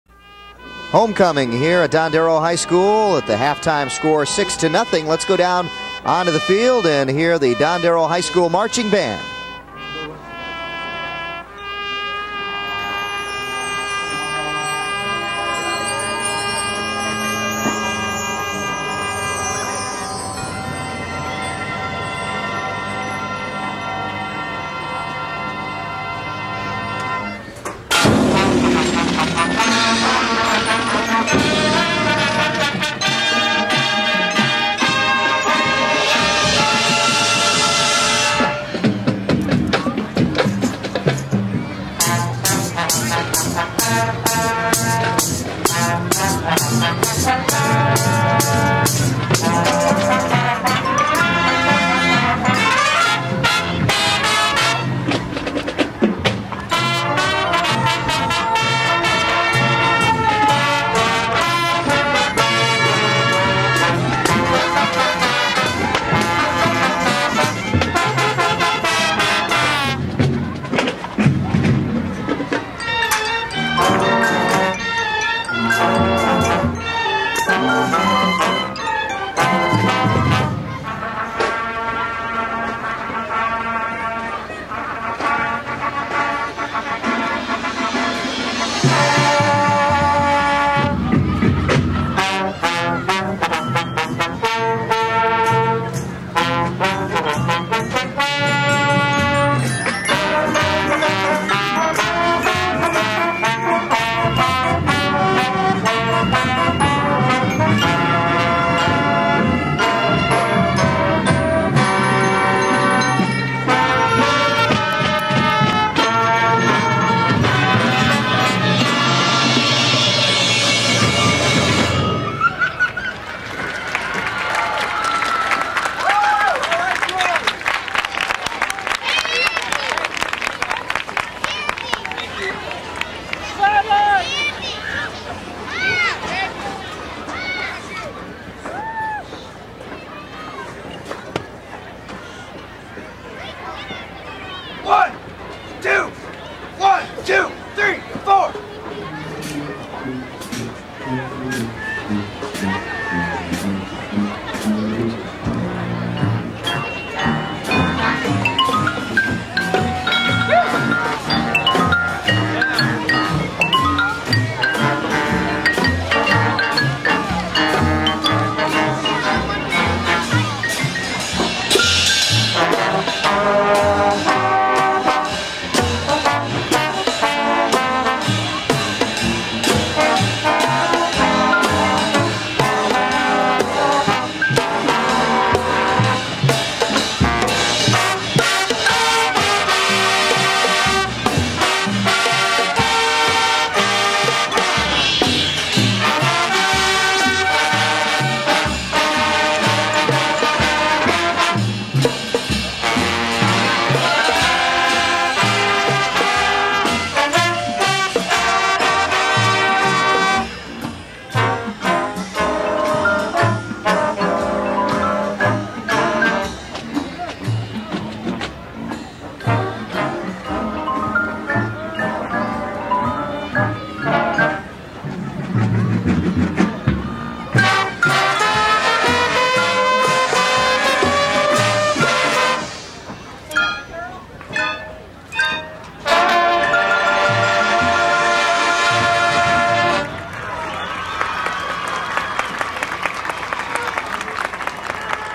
1991 Dondero High School Marching Band Homecoming Halftime Show vs Adams from United Artists
Dondero High School Marching Band
Royal Oak, Michigan

1991
Football Halftime performance from a local access cable broadcast